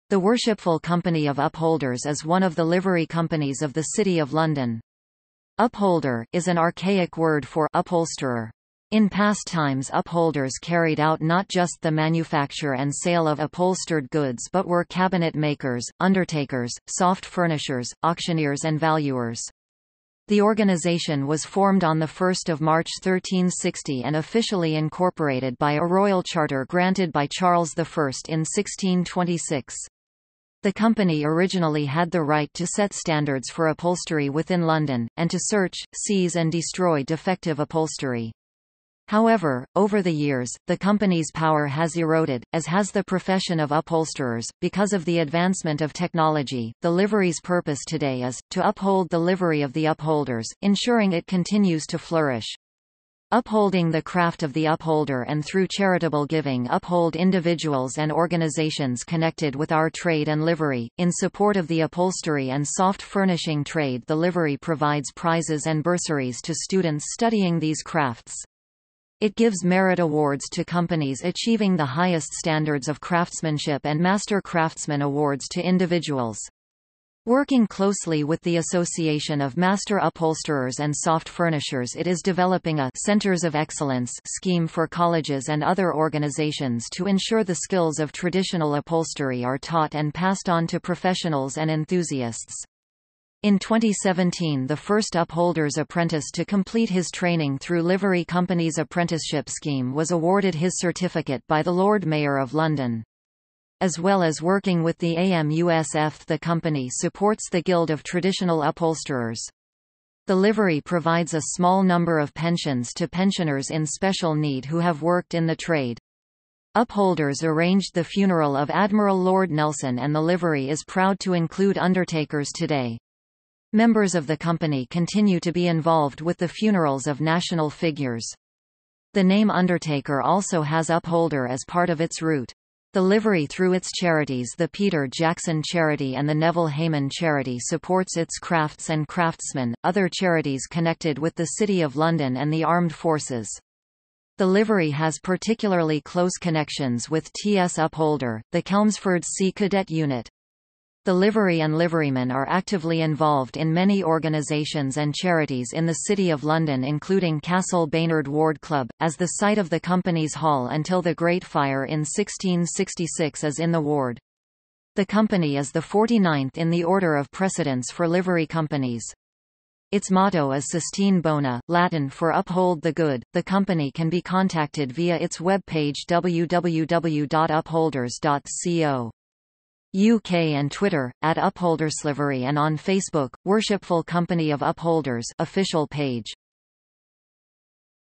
Worshipful Company of Upholders | Wikipedia audio article
- Socrates

SUMMARY
The Worshipful Company of Upholders is one of the Livery Companies of the City of London. "Upholder" is an archaic word for "upholsterer". In past times upholders carried out not just the manufacture and sale of upholstered goods but were cabinet makers, undertakers, soft furnishers, auctioneers and valuers. The organisation was formed on 1 March 1360 and officially incorporated by a Royal Charter granted by Charles I in 1626. The Company originally had the right to set standards for upholstery within London, and to search, seize and destroy defective upholstery. However, over the years, the Company's power has eroded, as has the profession of upholsterers, because of the advancement of technology.The Livery's purpose today is:

To Uphold the livery of the Upholders, ensuring it continues to flourish. Upholding the craft of the Upholder and through charitable giving uphold individuals and organisations connected with our trade and Livery.In support of the upholstery and soft furnishing trade the Livery provides prizes and bursaries to students studying these crafts. It gives Merit Awards to companies achieving the highest standards of craftsmanship and Master Craftsman awards to individuals. Working closely with the Association of Master Upholsterers and Soft Furnishers it is developing a "Centres of Excellence" scheme for colleges and other organisations to ensure the skills of traditional upholstery are taught and passed on to professionals and enthusiasts. In 2017 the first Upholders' apprentice to complete his training through Livery Companies' Apprenticeship Scheme was awarded his certificate by the Lord Mayor of London. As well as working with the AMUSF the Company supports the Guild of Traditional Upholsterers. The Livery provides a small number of pensions to pensioners in special need who have worked in the trade.
Upholders arranged the funeral of Admiral Lord Nelson and the Livery is proud to include undertakers today. Members of the company continue to be involved with the funerals of national figures. The name undertaker also has Upholder as part of its root.
The Livery through its charities the Peter Jackson Charity and the Neville Hayman Charity supports its crafts & craftsmen, other charities connected with the City of London and the armed forces. The Livery has particularly close connections with TS Upholder, the Chelmsford Sea Cadet unit.
The Livery and Liverymen are actively involved in many organisations and charities in the City of London including Castle Baynard Ward Club, as the site of the Company's Hall until the Great Fire in 1666 is in the Ward. The Company is the forty-ninth in the order of precedence for Livery Companies. Its motto is Sustine Bona, Latin for Uphold the Good.The Company can be contacted via its web and Twitter: @UpholdersLivery and on Facebook: Worshipful Company of Upholders (official page)